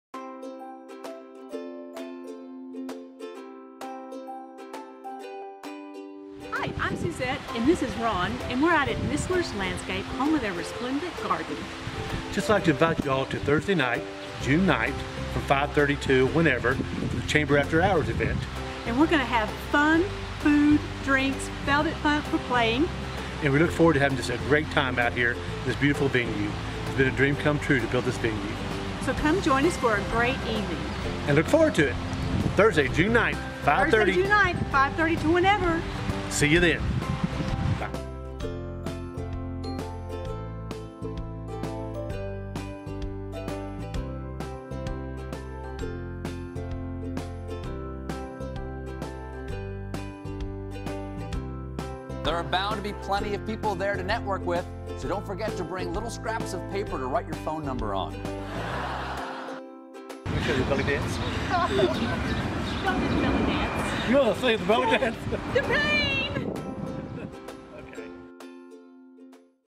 After Hours at Nistler's Invitation 6-9-16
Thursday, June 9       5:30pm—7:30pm
The Resplendent Garden at Nistler’s Lawn & Landscape
11701 Truman (East of S Osage on the Claude Highway)

Current and Future Chamber Members & Guests are invited to join us for another FREE Business Networking event.  There will be
hors d’oeuvres, drinks, music & door prizes.  Adults only please.  Questions?